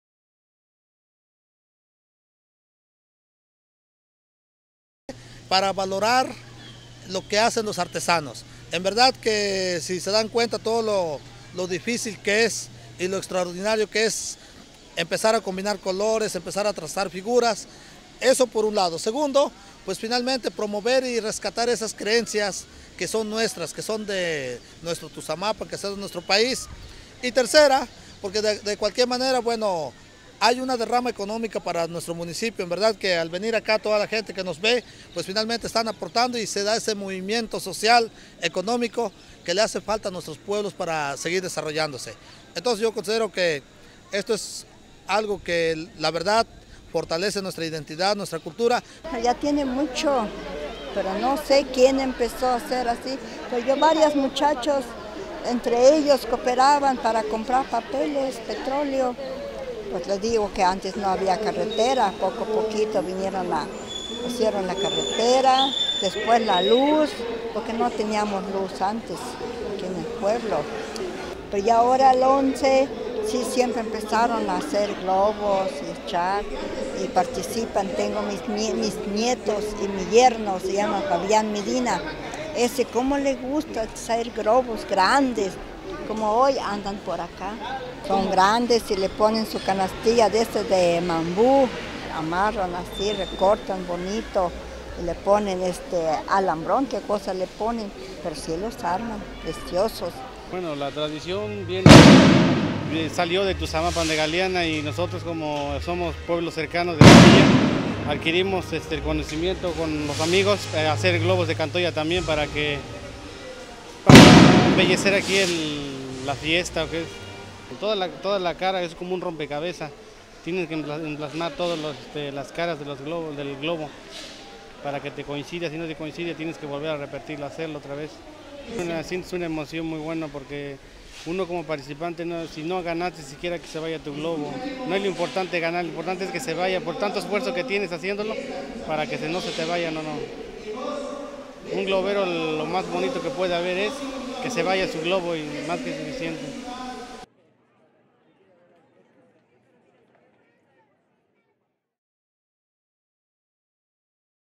Globos de cantolla, Tuzamapan de Galeana/ Subterráneos
Globos de cantolla, Tuzamapan de Galeana
Pablo Spencer/ Subterráneos
Tuzamapan de Galeana, Puebla. Del 14 al 16 de noviembre se realiza en esta región del estado, ubicada en la sierra nororiental, a los límites con el estado de Veracruz, el festival de globos de cantolla.
La tradición tiene aproximadamente 45 años de historia, durante los cuales se ha perfeccionado la construcción de estos enormes globos que son lanzados al cielo.
Los globos más sencillos están hechos con 6 pliegos de papel de china, pero estos pueden llegar a tener más de 5 mil pliegos y medir unos 15 metros de altura.
Se realiza un diseño en una hoja de papel cuadriculado, se cortan los pliegos de distintos colores para que representen cuadrados exactos y se van acomodando dependiendo el diseño, los papeles se unen con resistol blanco, hilo y cinta adhesiva transparente, y se les eleva con el aire caliente proveniente de una flama.
Durante el día se pueden apreciar cambios en el clima, que pasa de nublado-lluvioso a cálido y soleado, por lo cual todo debe estar preparado para que al escampar se lleve a cabo el lanzamiento de la mayor cantidad de globos posibles.